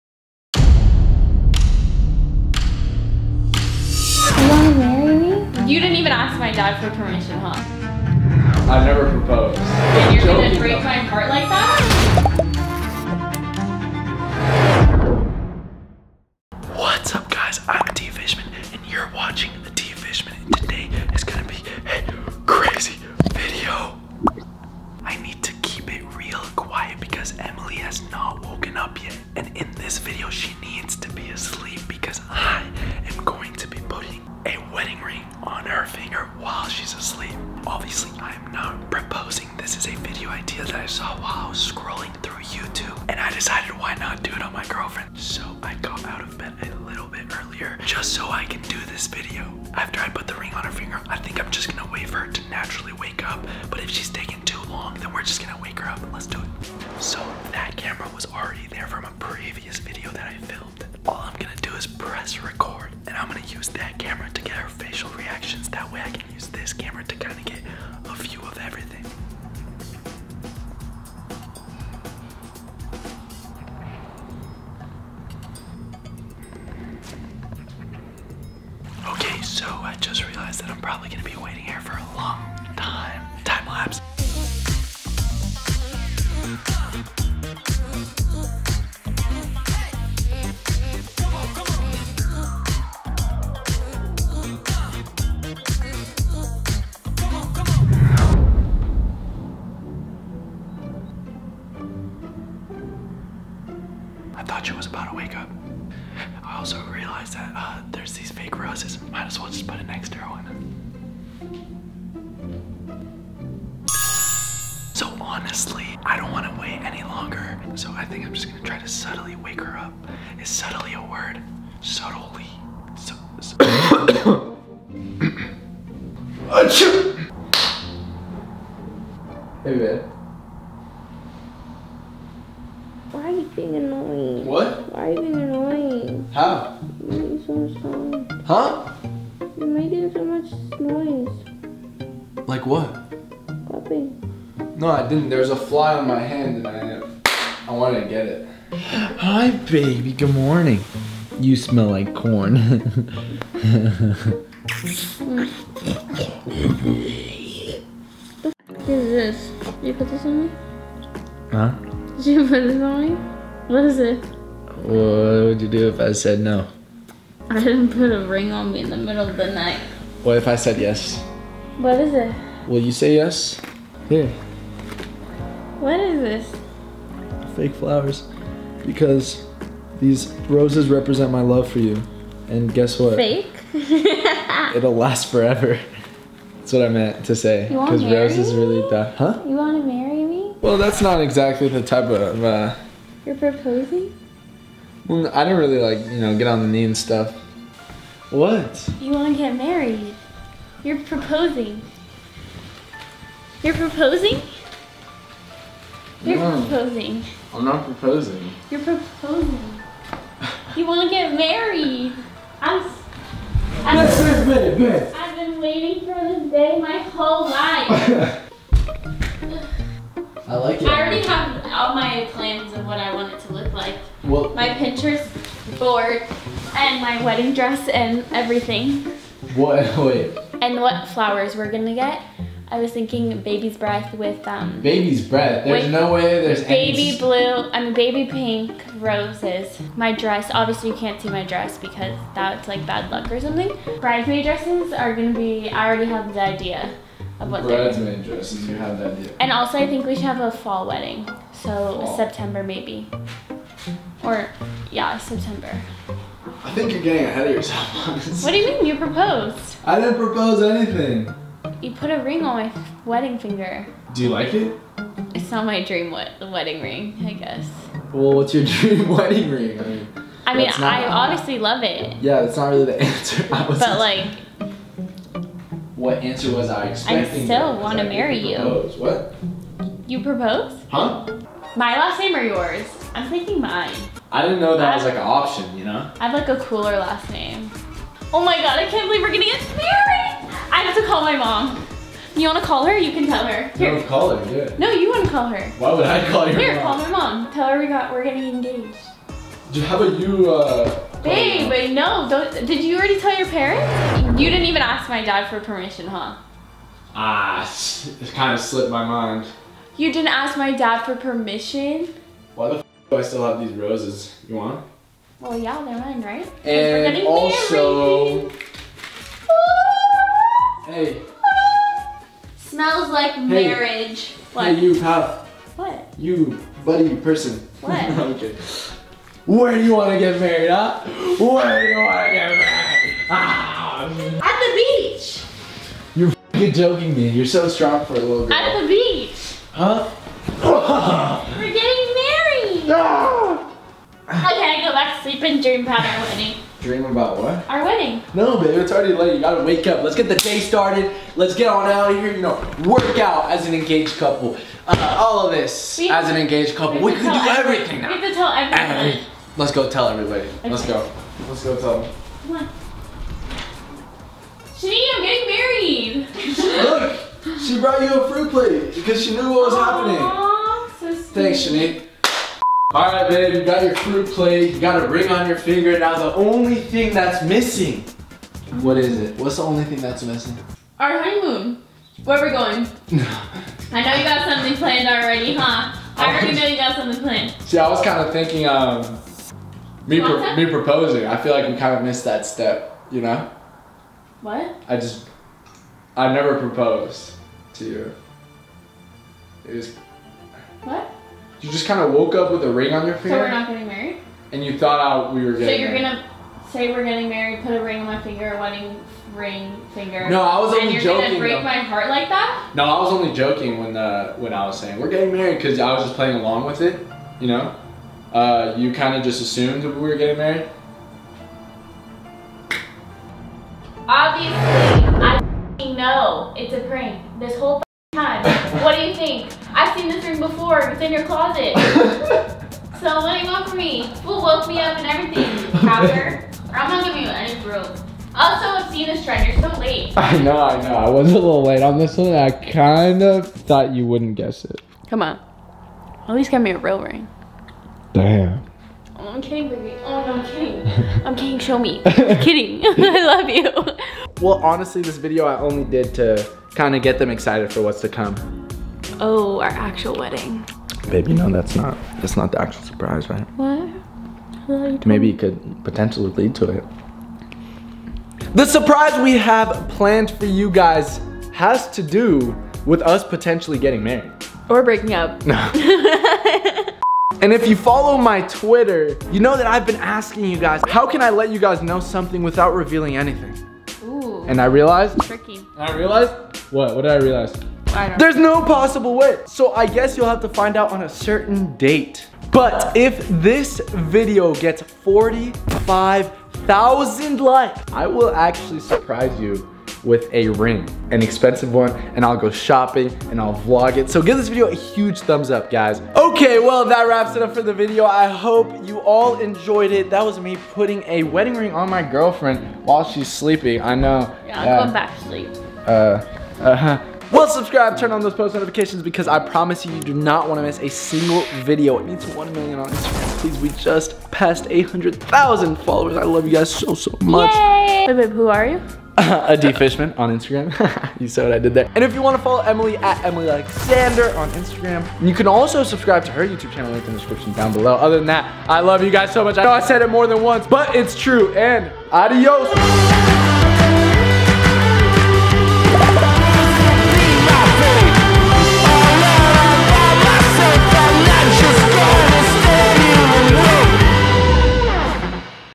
Putting A Wedding Ring On Her Finger While She's Asleep.. *Cute Reaction*
Hey Addicts! In this video, I saw this trend on Youtube where boyfriends put wedding rings on their girlfriends while they are still asleep! I decided to do that on Emily and it was well worth it.. Her reaction was priceless! Watch to see what went down..